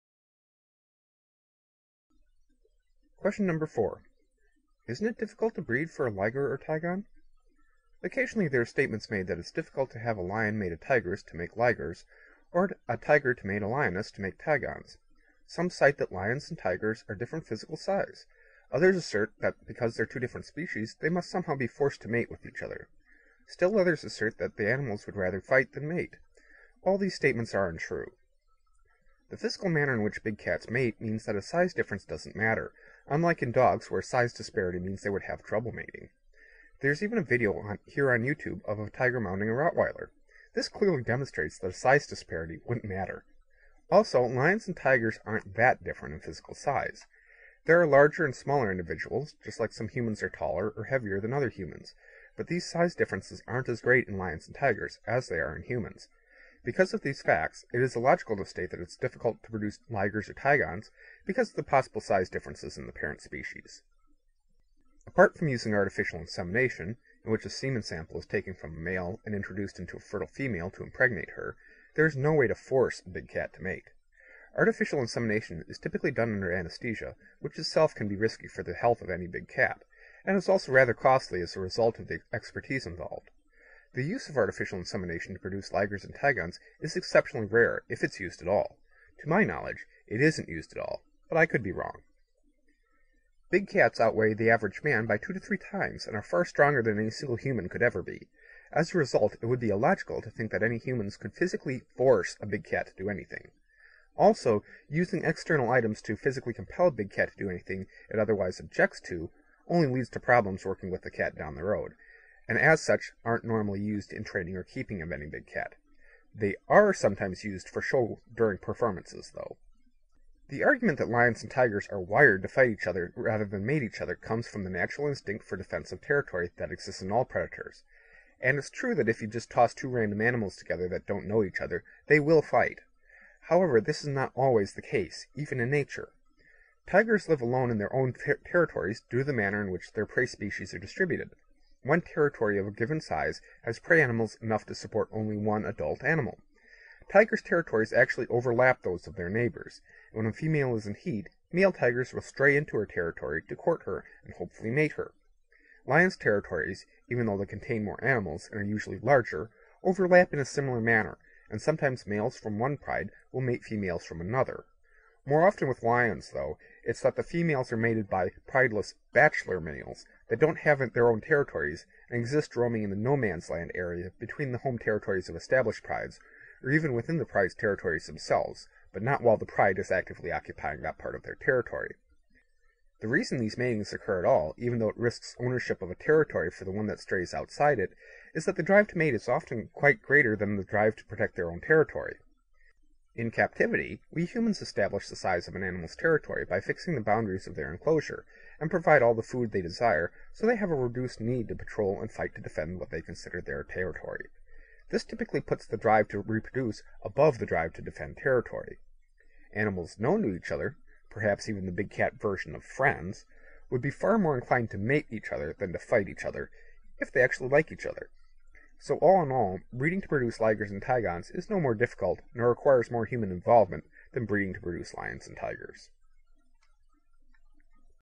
Ligers and Tigons - Separating Fact from Fiction - Question 4
Ligers and Tigons: Separating Fact from Fiction

Question 4: "Isn't it difficult to breed for a liger or tigon?"

There are statements made that assert that somehow it is difficult to breed lions to tigers, or that it requires some forcable human manipulation. This video addresses issues relating to that subject.